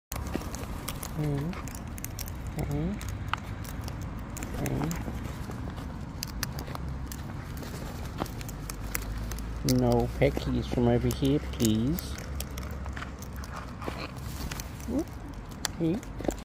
Patting a wild cockatoo!
Cheeky wild cockatoo lands on my head